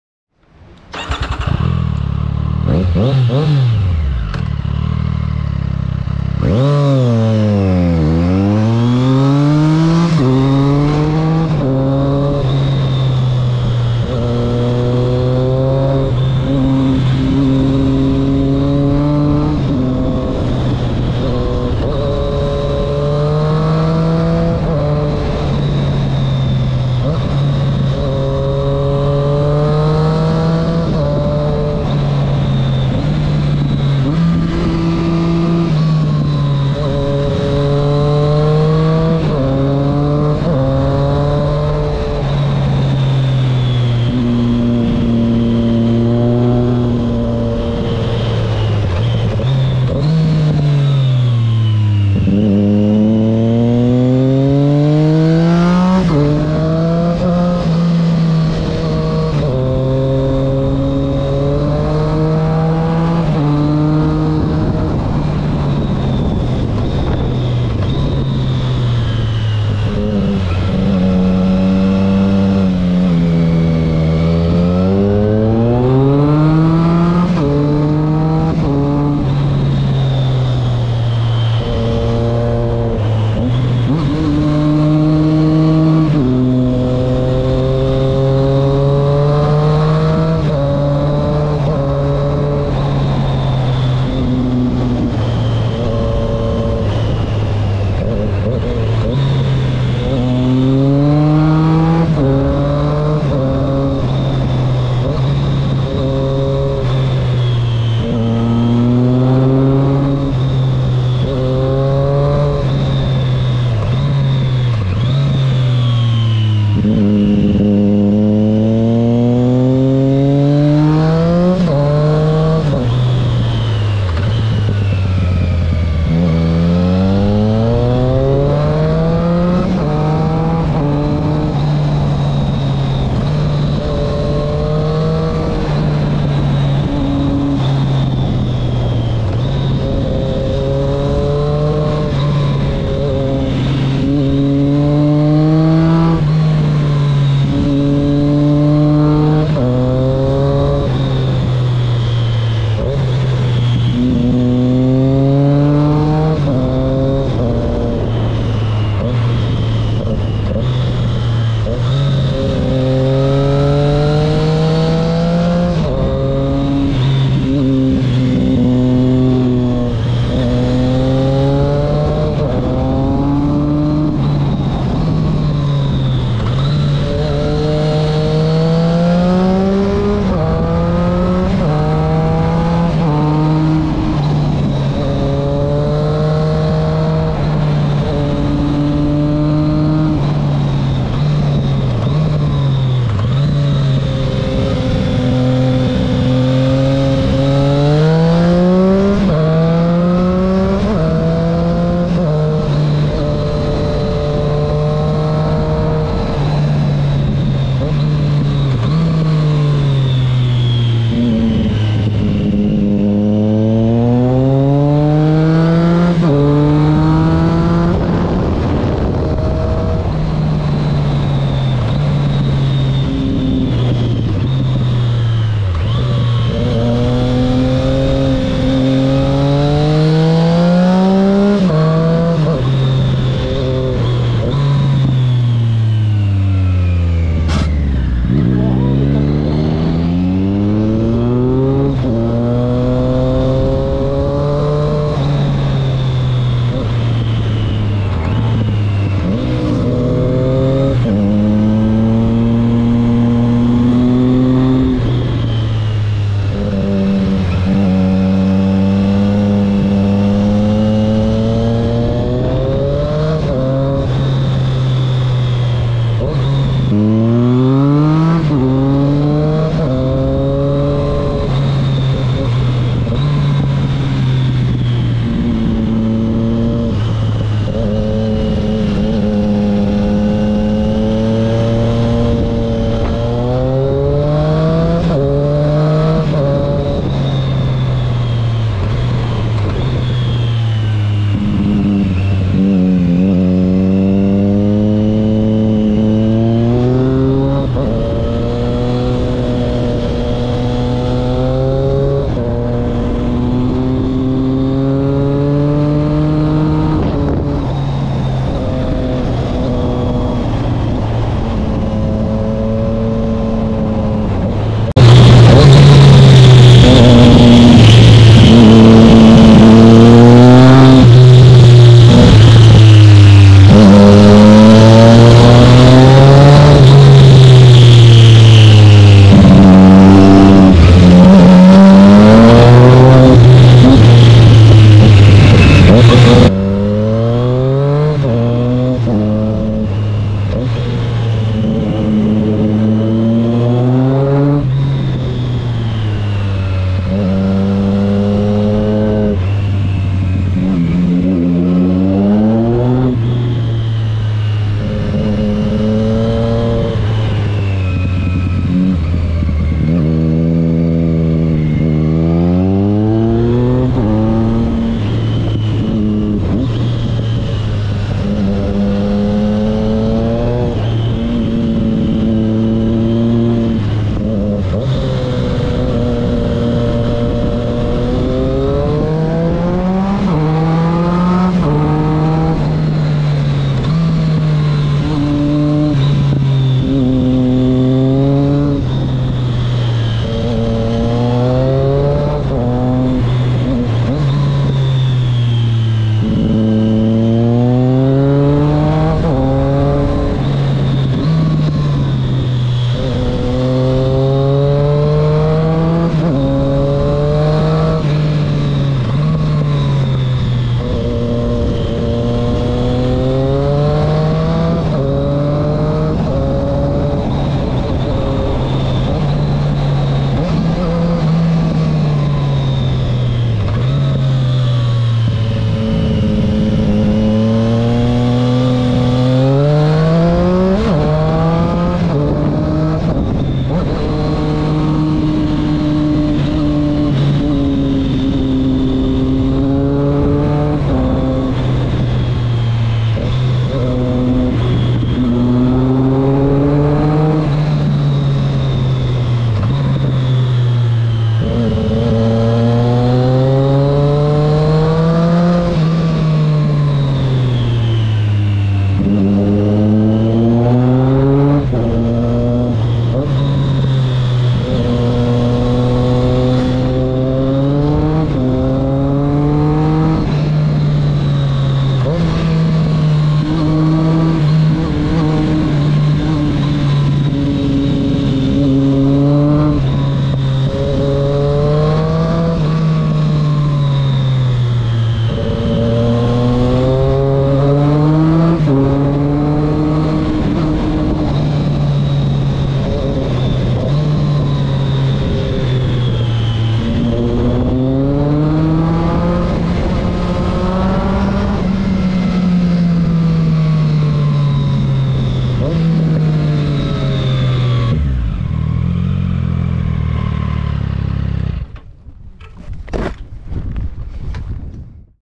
RIDE POV - STREET TRIPLE S - SC PROJECT EXHAUST⚡️
Voici ma street triple 660 S de 2020 bidé A2
Instgram : un_medecin_a_moto
Ride Safe V !